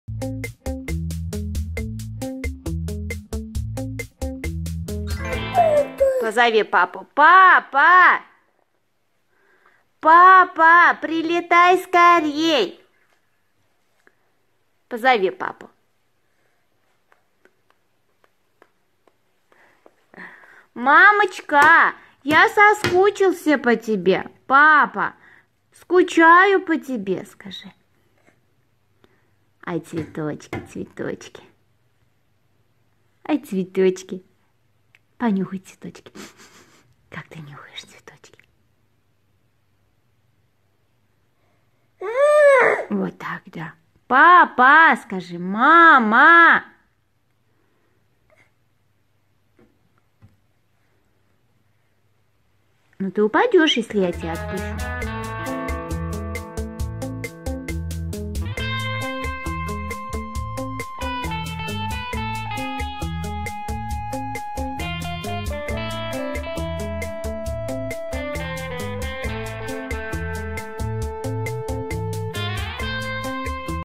ПЕРВОЕ ВИДЕО - СЫН КСЕНИИ СОБЧАК (ondom2.com)
В этом выпуске новости и слухи  про сына бывшей ведущей дома 2 Ксении Собчак и её мужа Максима Виторгана.